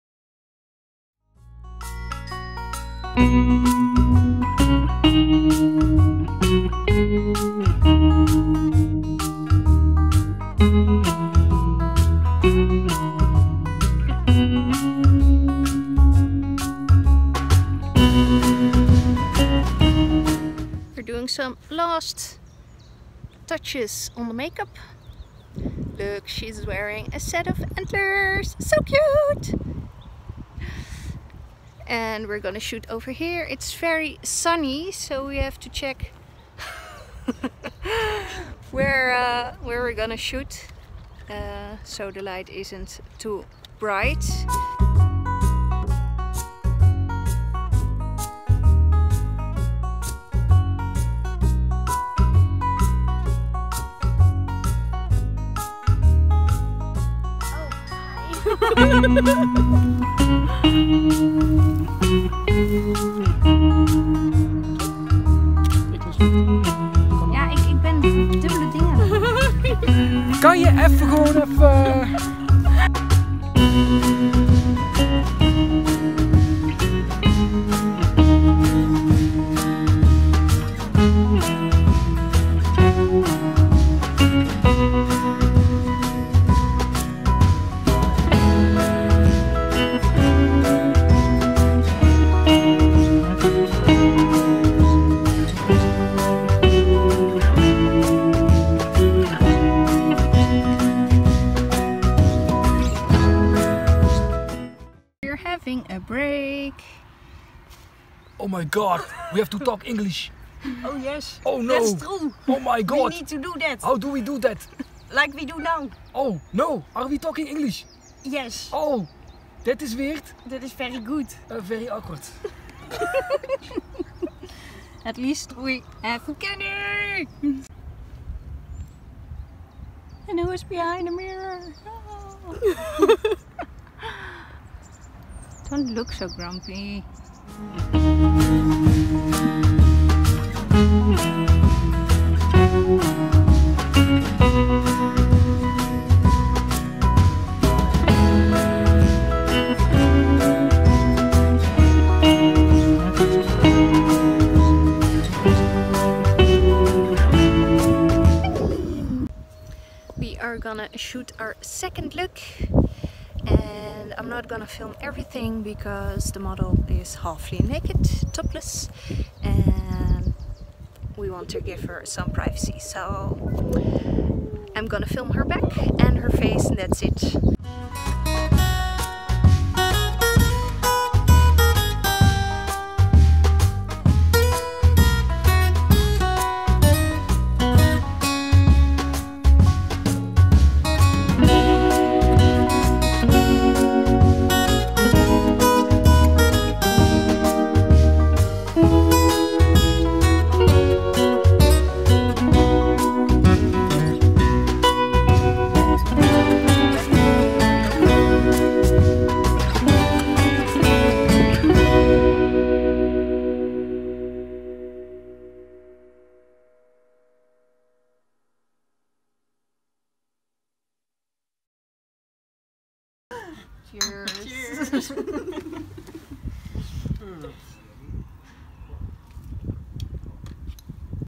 BTS // Forest spirit
In spring 2018 Maskarada did a photoshoot with Jaap Ian Tessler and model Daphne. After a long wait I can now share with you the behind the scenes video!
Location; De Heerlijkheid - Eck en Wiel (the Netherlands)